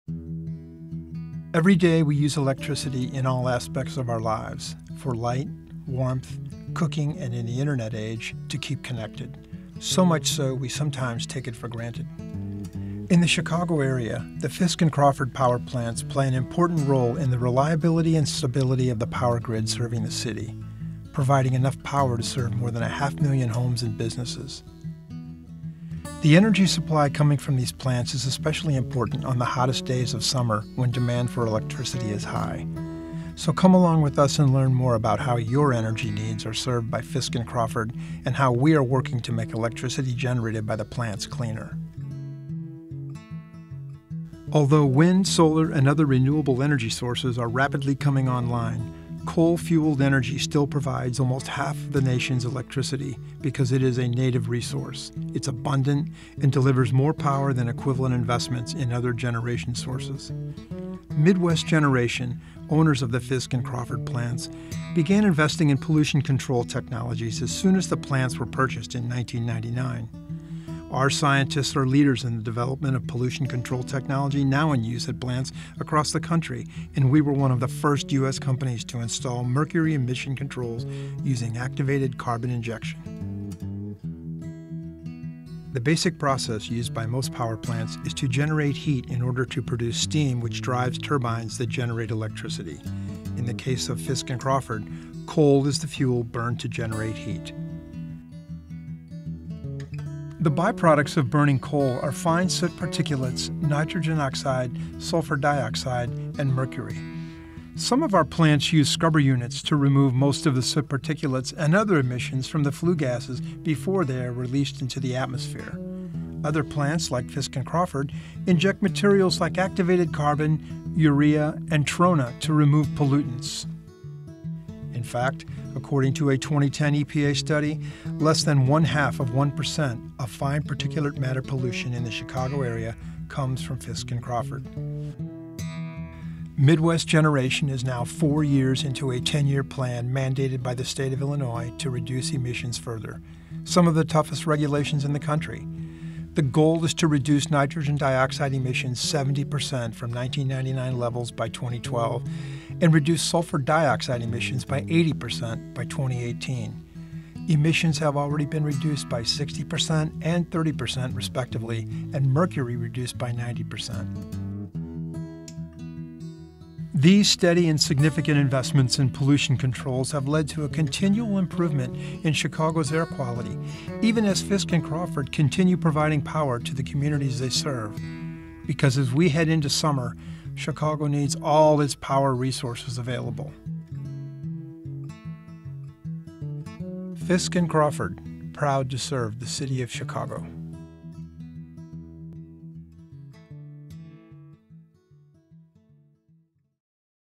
Virtual Tour of the Fisk and Crawford Power Plants
Take a tour of Midwest Generation's Chicago-based Fisk and Crawford power plants and find out how they generate electricity while meeting tough state and federal emissions regulations.